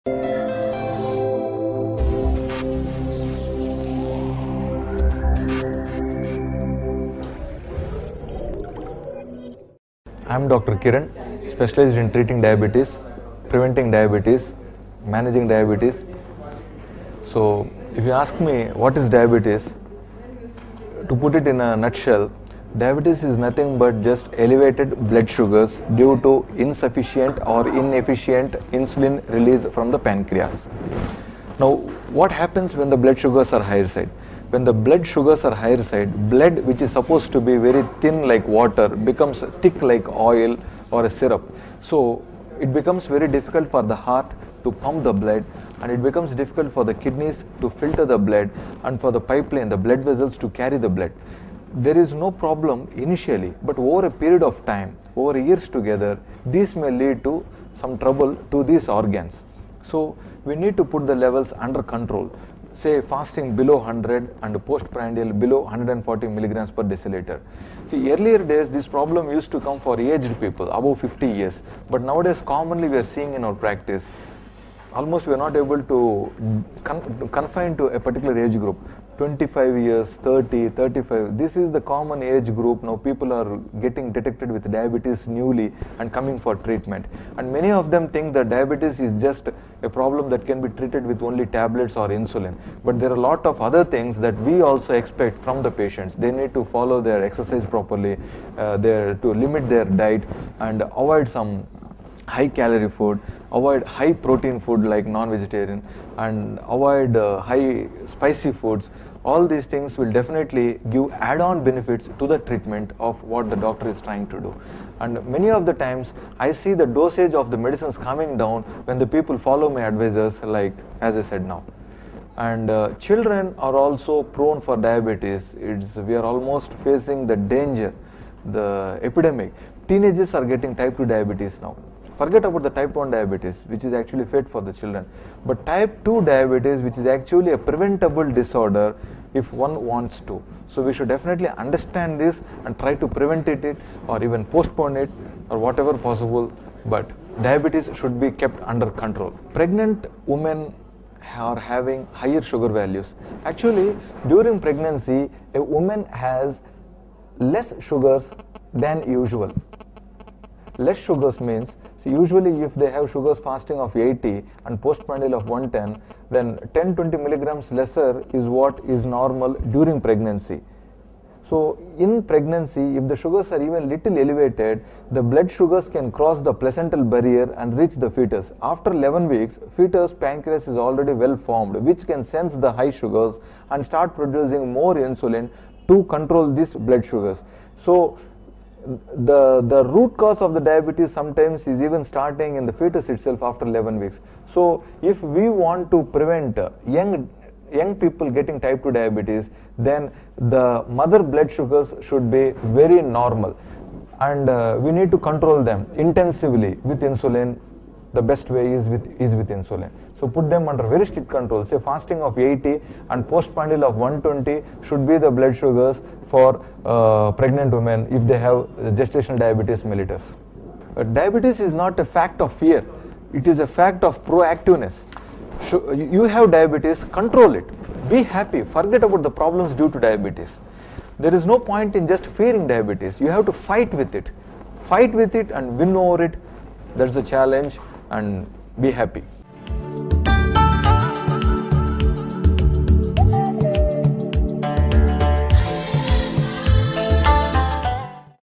Kiran, Dr. Kiran's Diabetes, Clinical Research Institute
Dr. Kiran, Dr. Kiran's Diabetes, Clinical Research Institute speaks in an exclusive interview with hybiz.tv I am specialist in preventing and treating diabetes. Diabetes is elevated blood sugars due to insufficient and inefficient insulin released from pancreas. When the blood sugars are on the higher side blood which is supposed to be thin like water becomes thick like oil. It becomes difficult for the heart to pump blood and it becomes difficult for the kidneys to filter the blood and the blood vessels to carry the blood. Patients need to exercise regularly, control their diet, avoid high calorie and protein food. Children are also prone to diabetes. Teenagers are affected with Type II diabetes. Pregnant women have high sugar values. Diabetes is the fact of pro activeness.